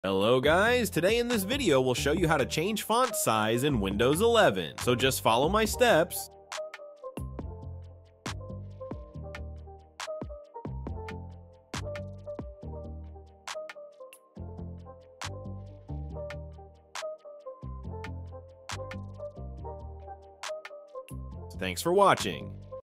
How To Change Font Size In Windows 11 - Easy Guide
You will learn step by step secret process to change font size in windows 11